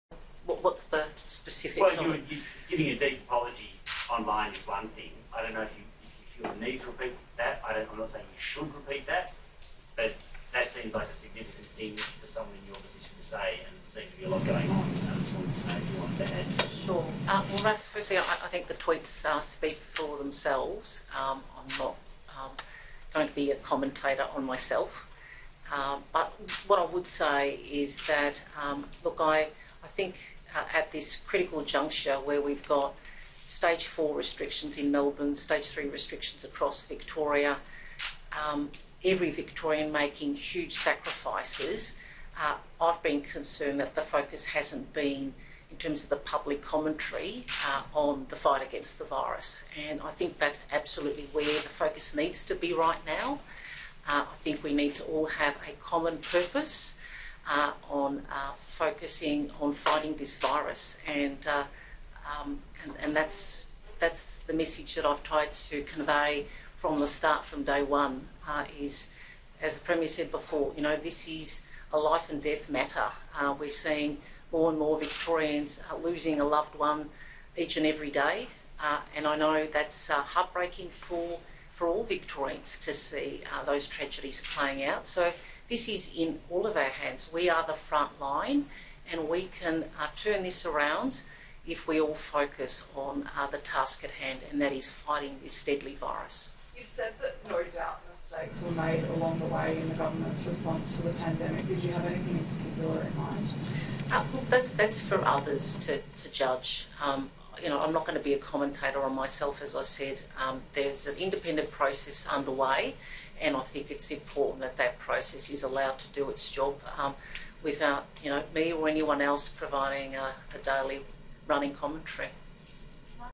My tweets ‘speak for themselves’: Mikakos
Victoria’s Health Minister Jenny Mikakos has refused to comment on her late-night weekend tweets in which she said she was “deeply sorry” if her effort to combat COVID-19 since its arrival in Australia in January was not enough.

“The tweets speak for themselves; I’m not going to be a commentator on myself,” she said at the media conference.

“But what I would say is that, look, I think at this critical juncture where we have got stage four restrictions in Melbourne, stage three restrictions across Victoria, every Victorian making huge sacrifices, I’ve been concerned that the focus hasn’t been, in terms of the public commentary, on the fight against the virus.

“This is a life and death matter, we’re seeing more and more Victorians losing a loved one.”

At about midnight on Saturday Ms Mikakos started a thread on Twitter.

“I’ve worked every day to keep everyone safe. I have put every ounce of energy I’ve had into that effort.  If it wasn’t enough, then I’m deeply sorry,” Ms Mikakos said.

“I’ve always striven to be upfront and measured about the challenges facing us. So it pains me to see the incorrect assumption made that somehow I can single-handedly report on the actions of countless individuals and many agencies involved in our pandemic response.

“Let the independent judge do her job, let the cards fall where they may.  I believe there is nothing to fear in seeking the truth. The truth will set you free.”